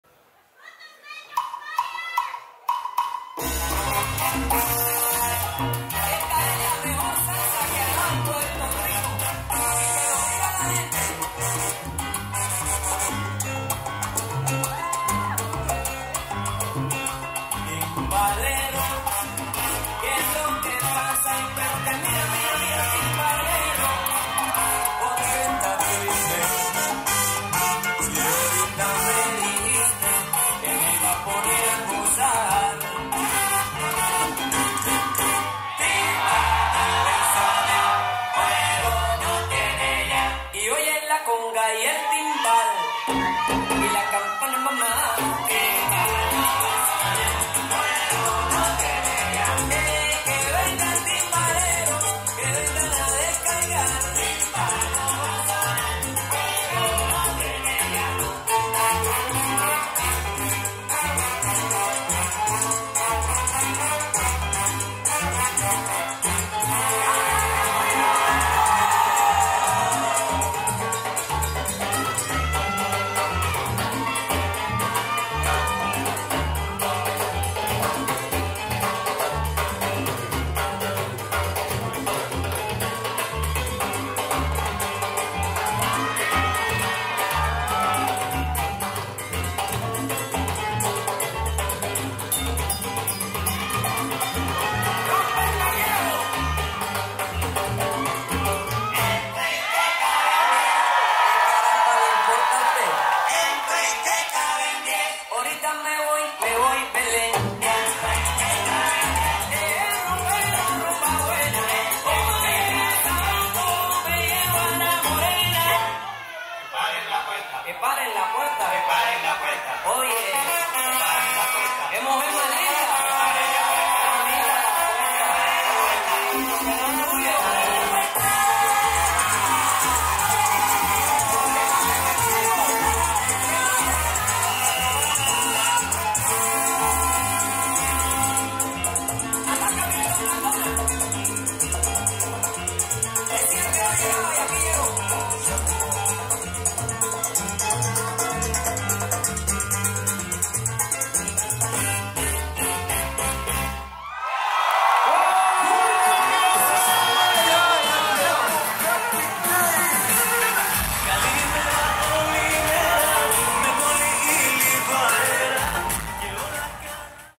Dimitris & Yolena - Salsa Show at World Stars Salsa Festival 2024, Albena / Bulgaria
Incredible show from Dimitris & Yolena at World Stars Salsa Festival 2024!
📍Next edition: 24-28 April 2025
📍Albena Resort, Bulgaria

Place: Albena Resort, Bulgaria
Video: Reign Mambo